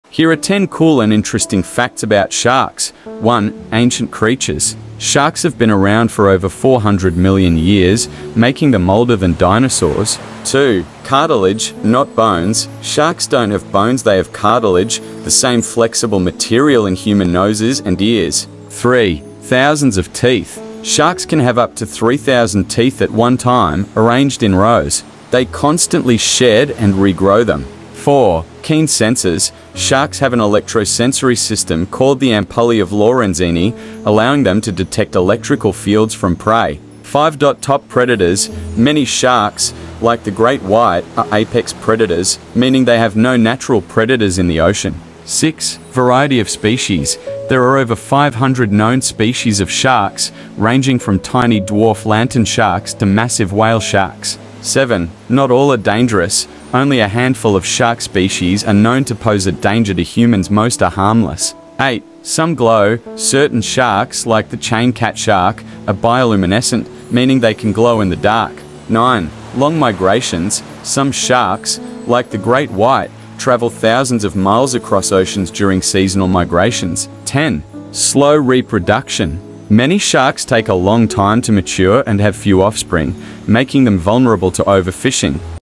10 Mind-Blowing Facts About Sharks You Didn’t Know!
Dive into the deep with these 10 amazing facts about sharks! From ancient history to glowing species, sharks are some of the most fascinating creatures in the ocean. Whether you’re a shark lover or just curious, this video will blow your mind with surprising shark science and trivia.